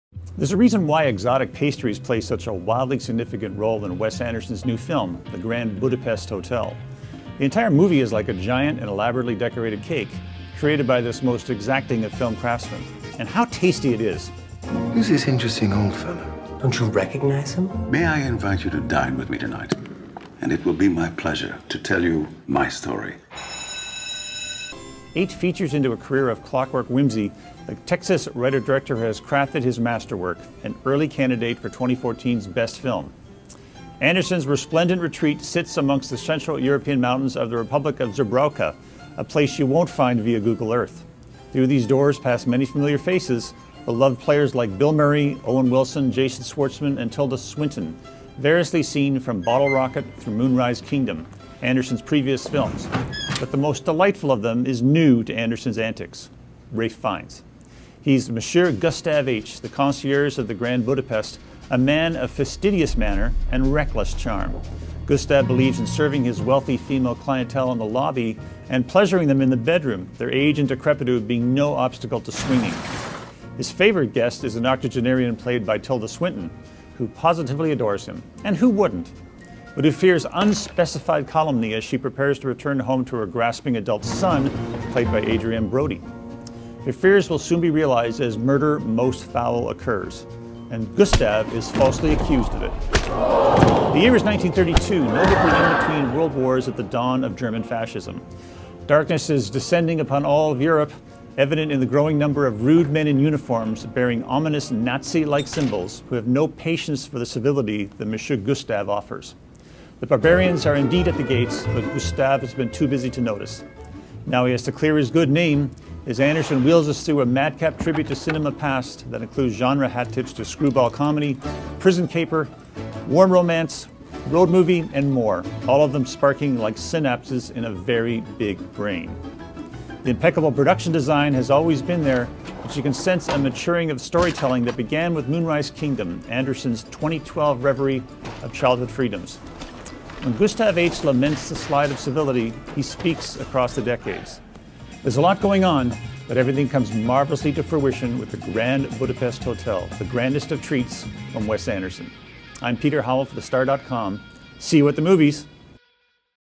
First Reel Movies: The Grand Budapest Hotel
Peter Howell reviews the latest Wes Anderson movie, a tasty treat from one of cinema's most exacting craftsmen.